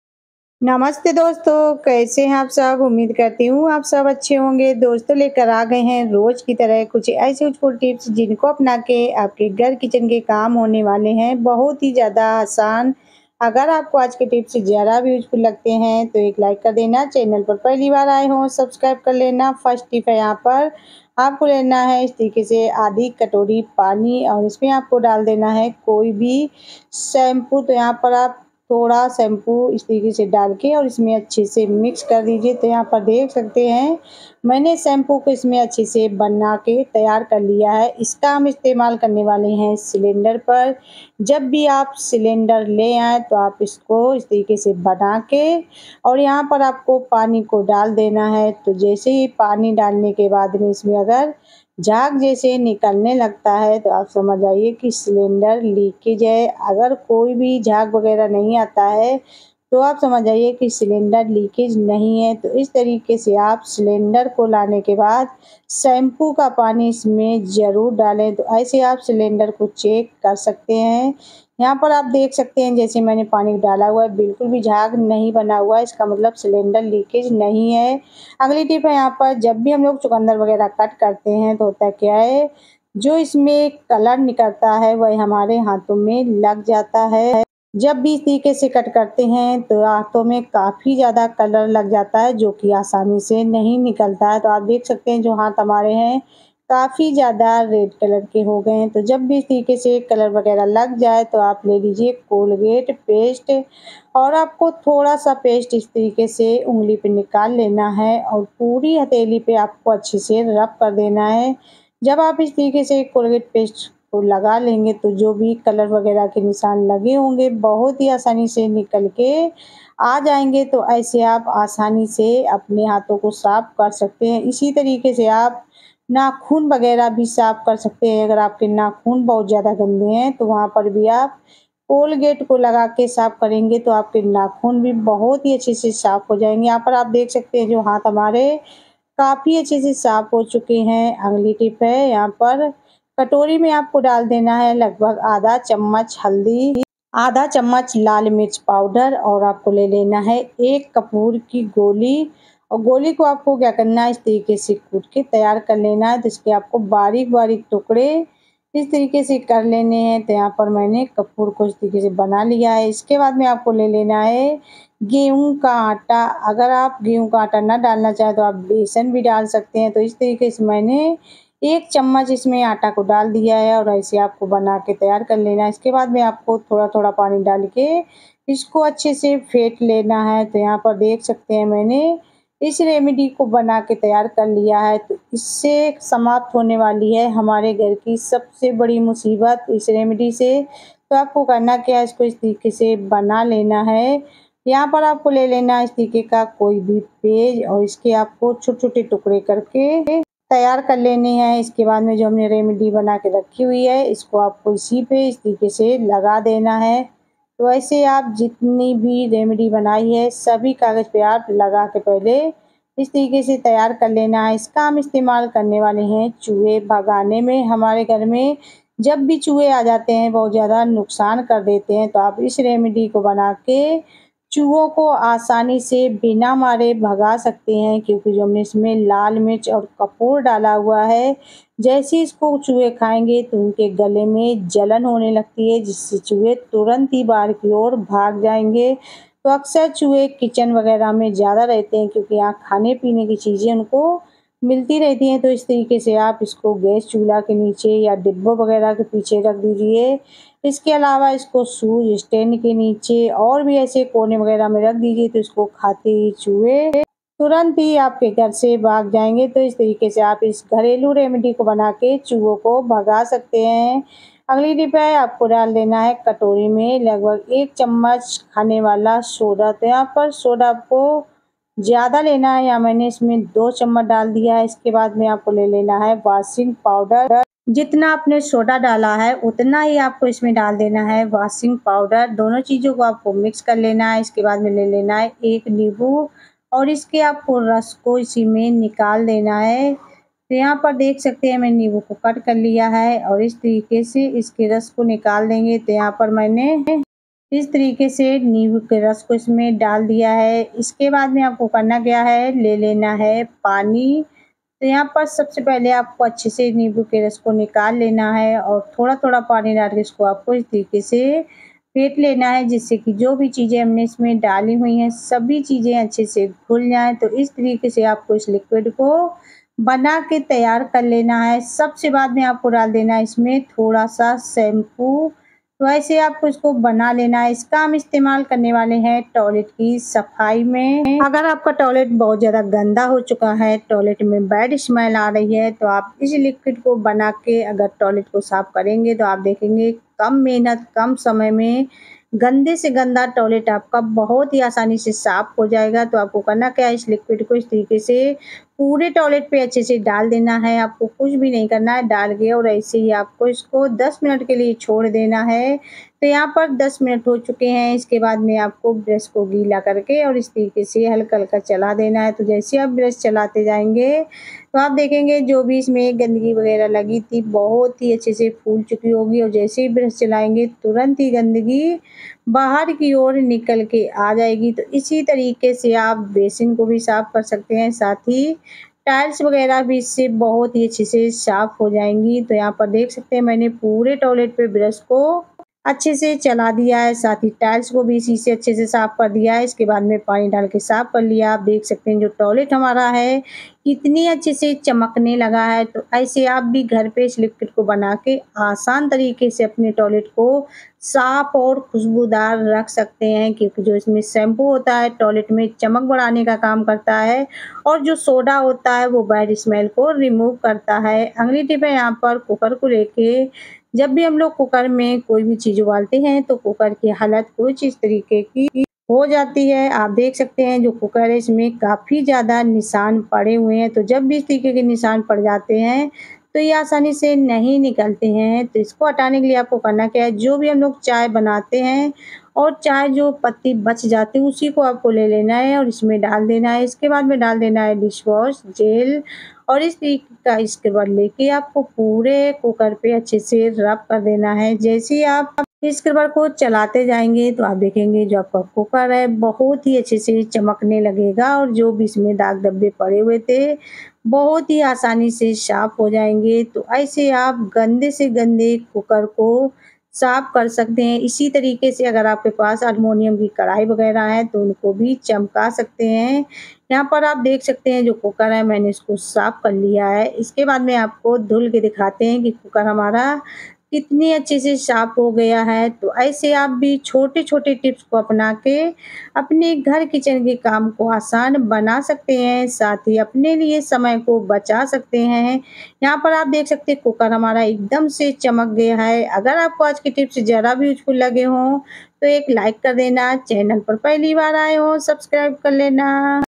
टॉयलेट में नींबू डालते ही गंदे से गंदा टॉयलेट होगा क्लीन और खुशबूदार # kitchen tips# cleaning tips
kitchen tips in Hindi useful kitchen tips# amazing kitchen tips toilet cleaning tips# money saving tips smart home maker all time useful tips# new tips